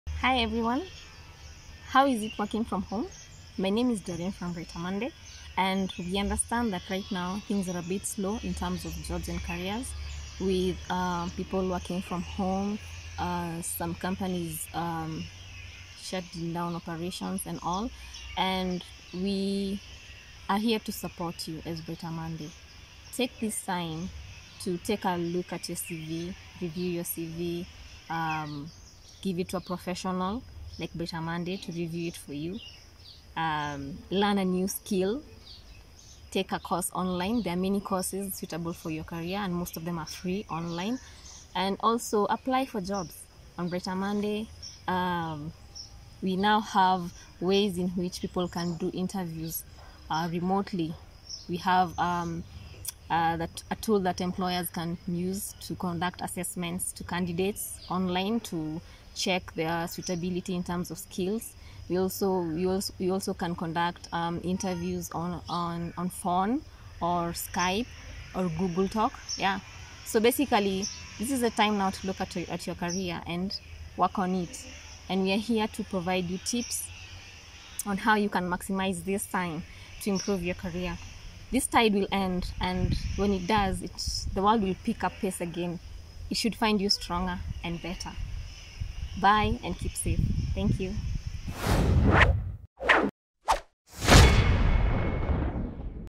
Covid-19 and Keeping Your Career Afloat: Practical Tips
Working from home and you suddenly have extra time in your hands?
You can use this downtime to;
1. Apply for job
2. Review your Cv
3. Learn a new skill
4. Take a course online